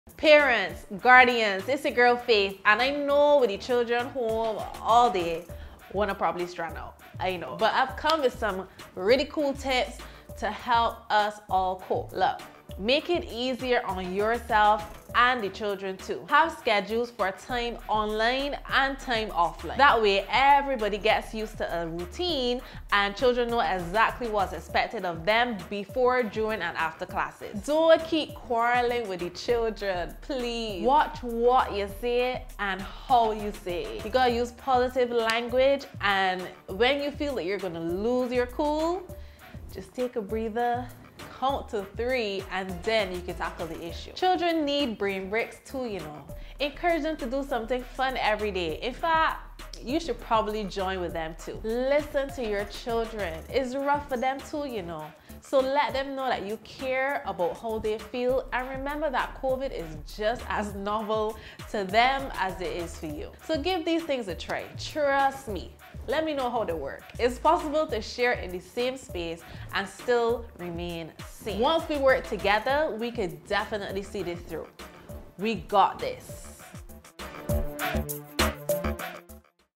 Coping With COVID 2
Entertainer Faith shares words of encouragement and tips with parents and guardians on coping with COVID-19.